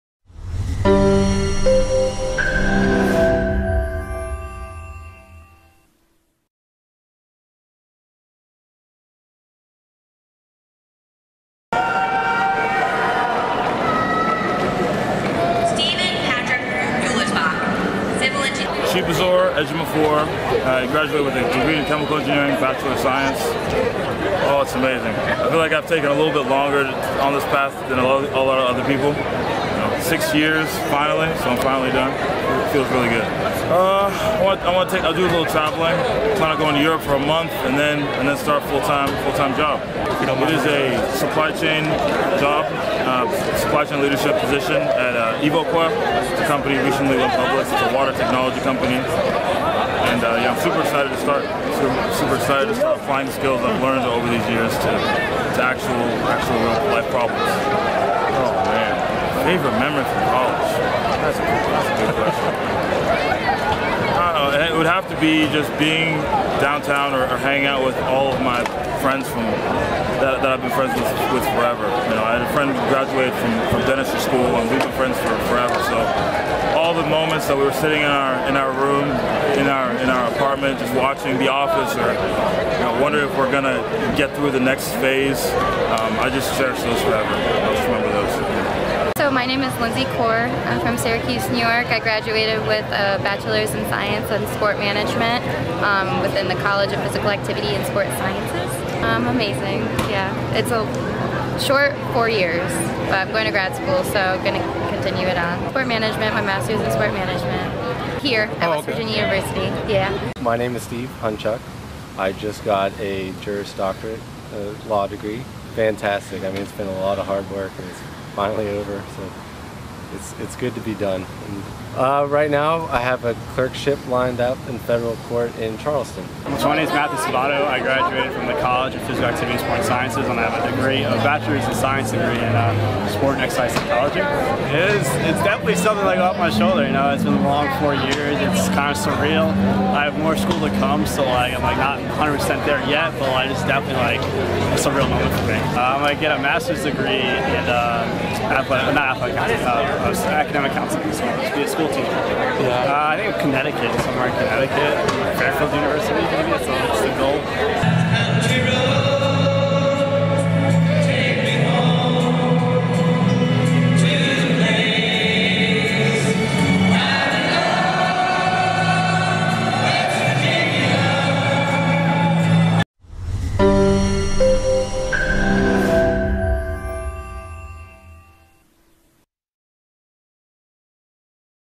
WVU May 2018 Commencement Highlights 5/13/18
More than 4,450 West Virginia University students graduated during Commencement ceremonies this year.